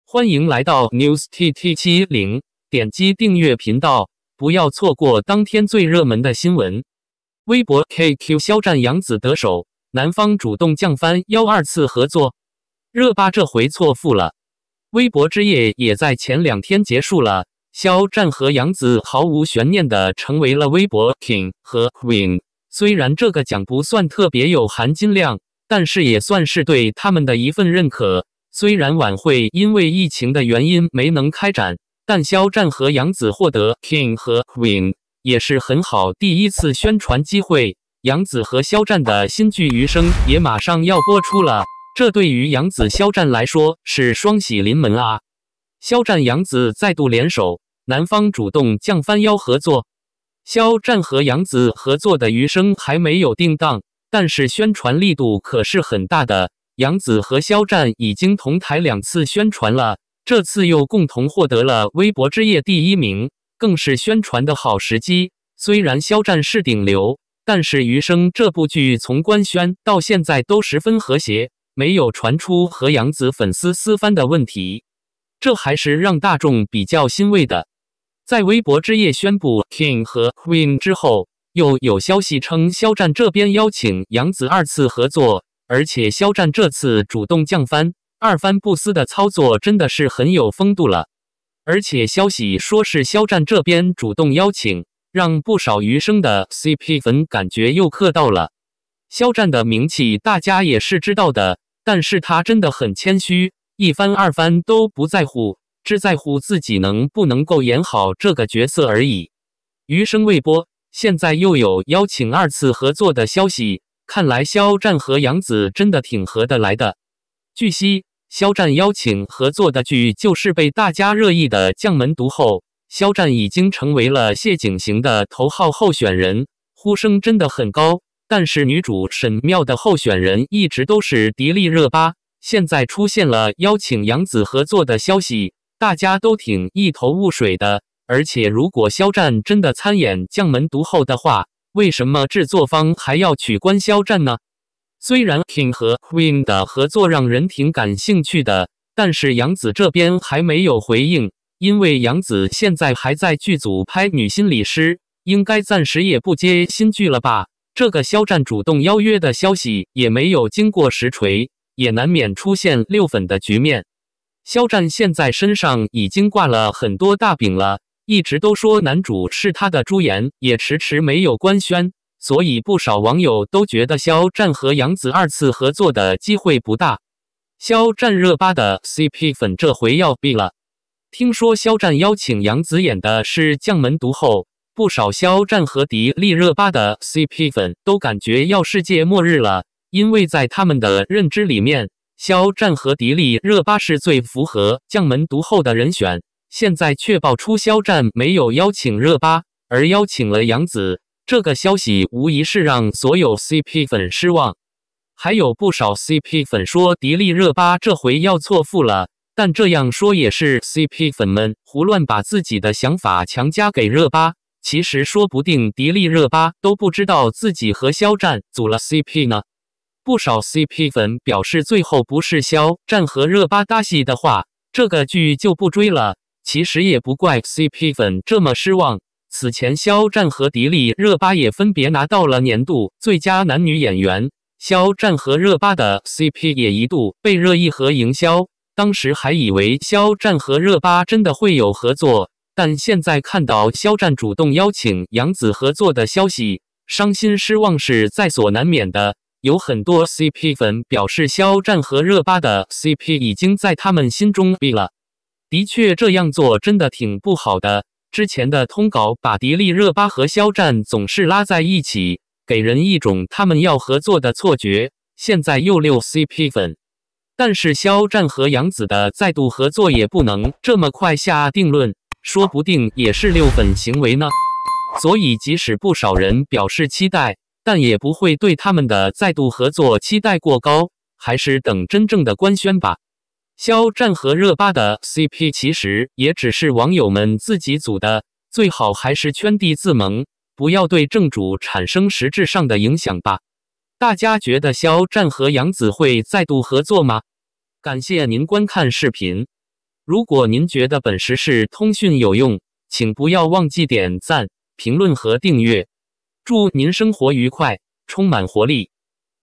ENG SUB - 微博KQ#肖战 杨紫 得手，男方主动降番邀二次合作？热巴这回错付了 【NEWS TT7012月04日】
微博KQ#肖战 杨紫 得手，男方主动降番邀二次合作？热巴这回错付了